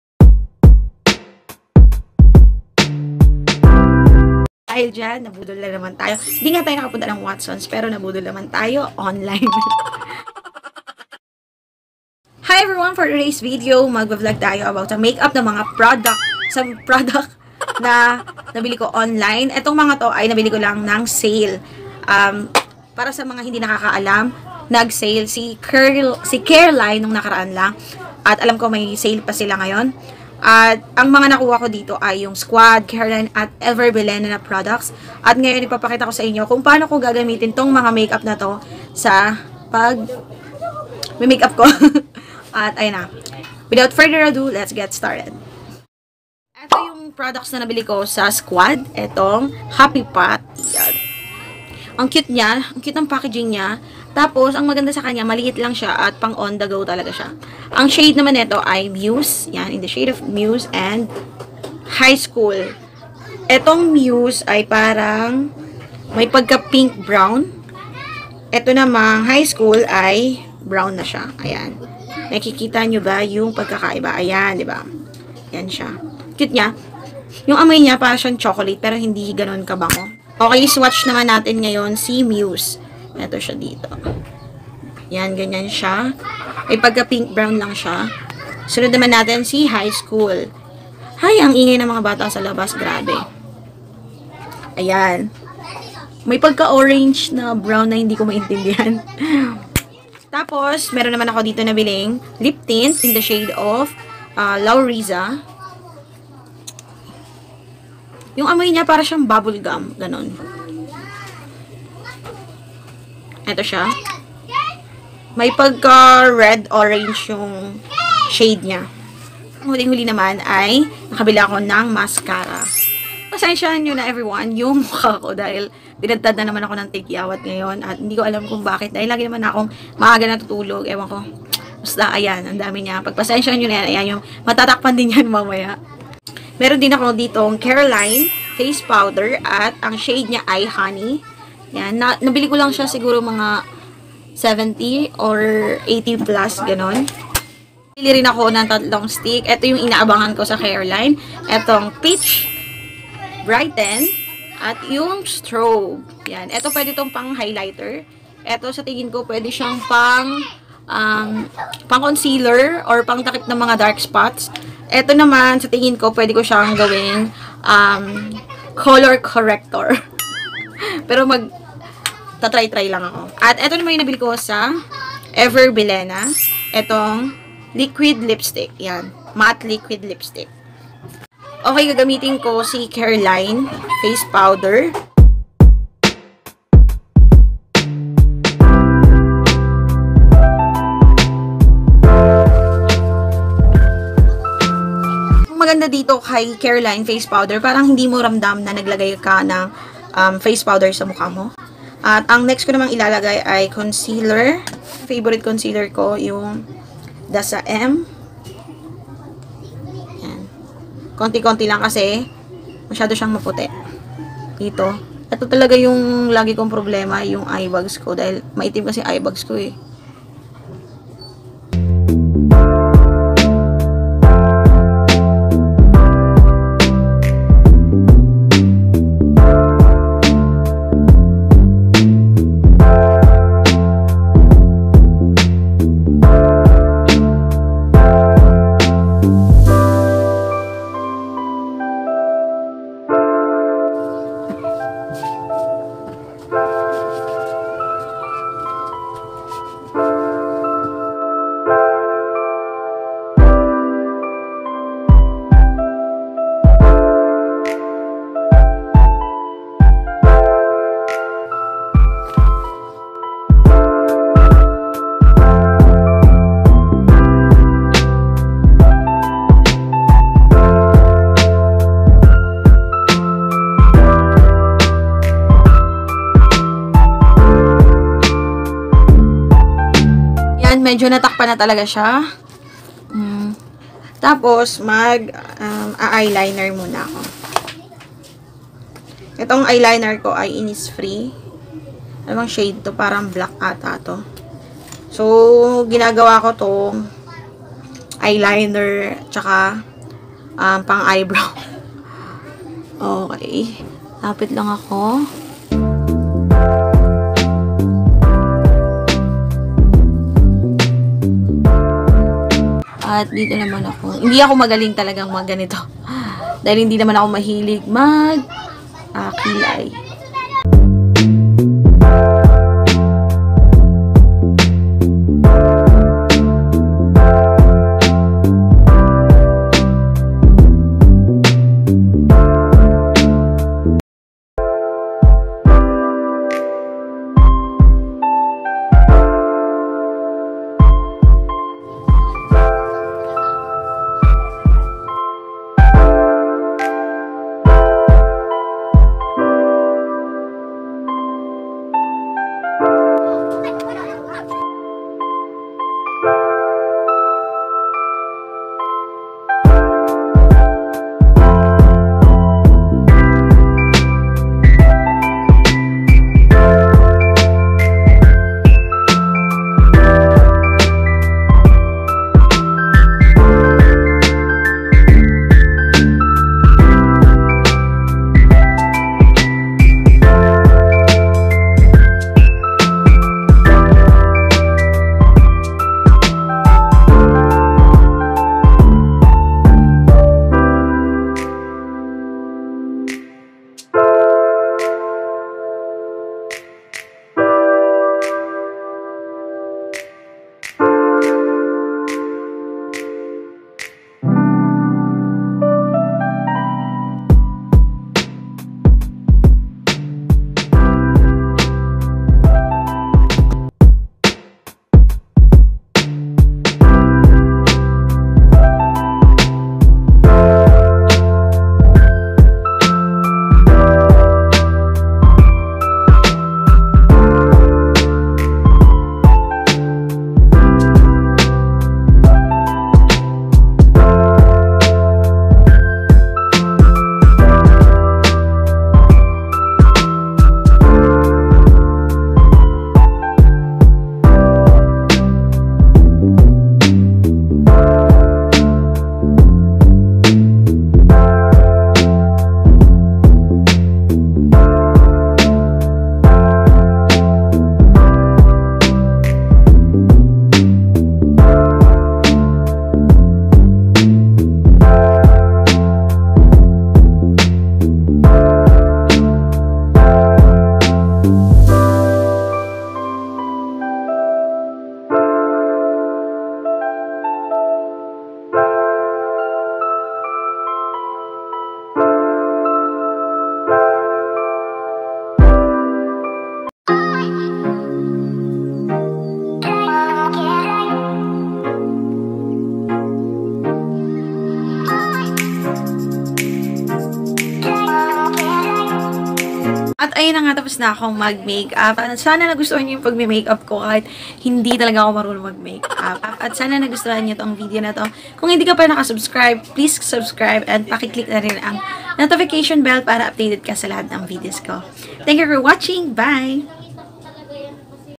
38 PESOS LANG MAY CARELINE PRODUCTS KA NA!! (careline, squad cosmetics, ever bilena)
hi everyone! pls don't judge me kung hindi ako magaling mag makeup hahaha!